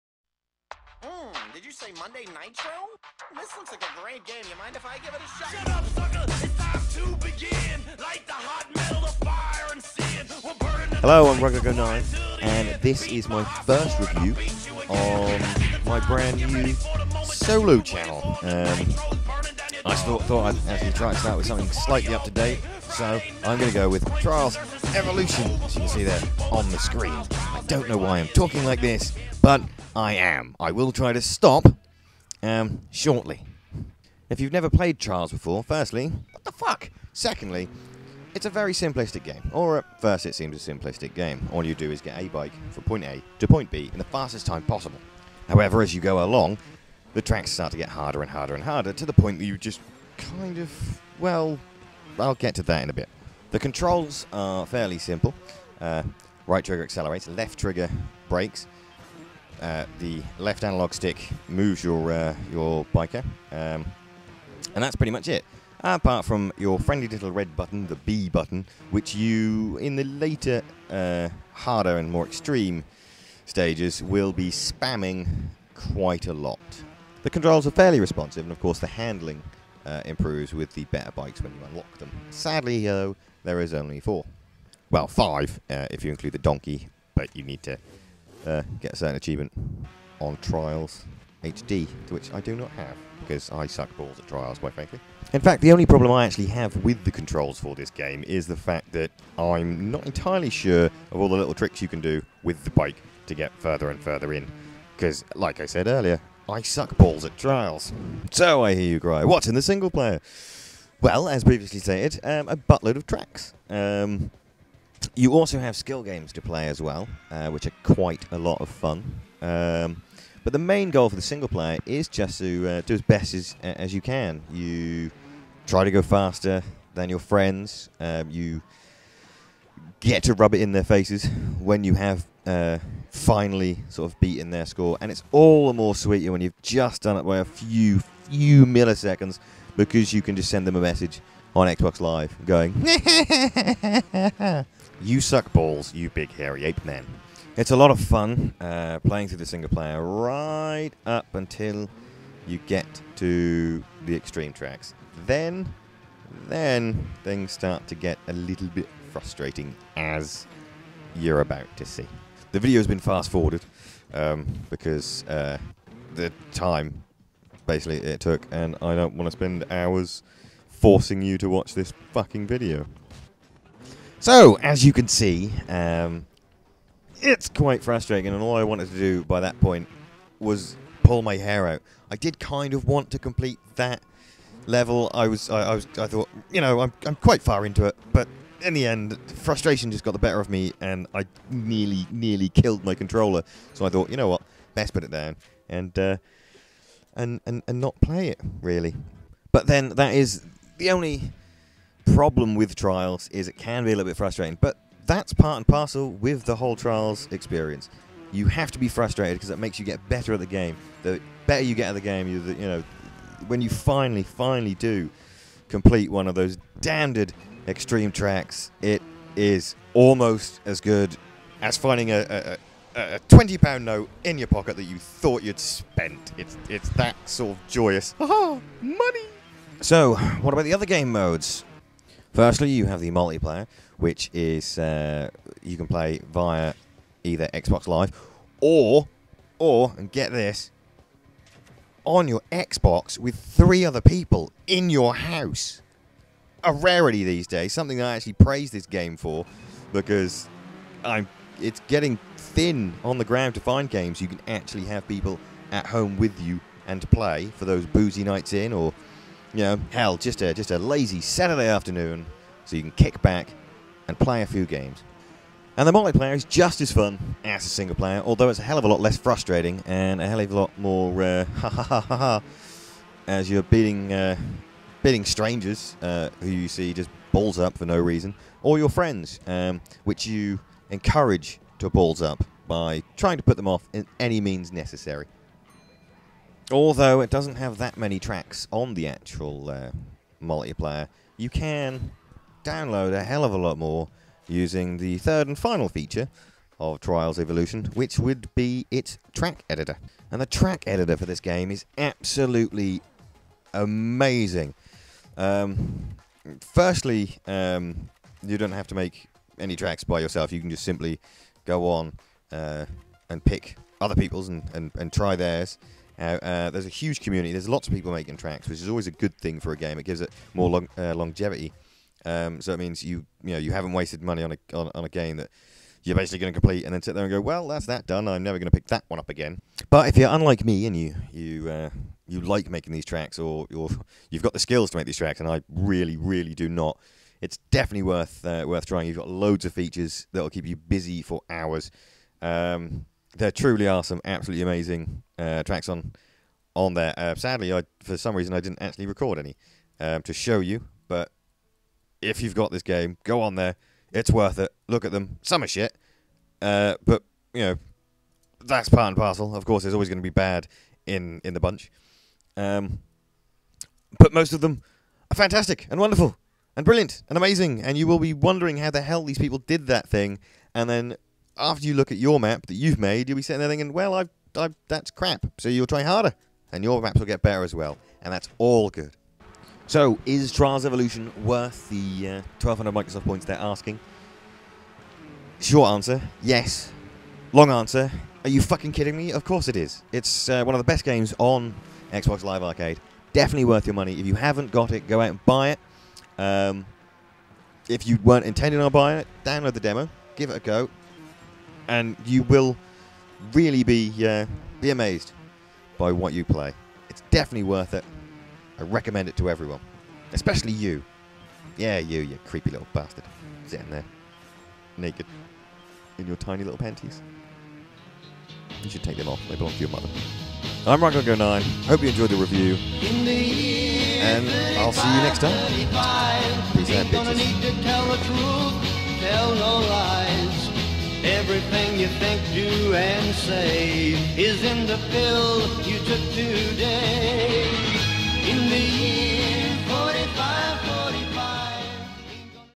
Unbiased Reviews. Trials Evolution
Rugdawg09 William Shatner's his way though his first review for his solo channel, Will Trials Evolution survive? Most likely because its a game and it's not alive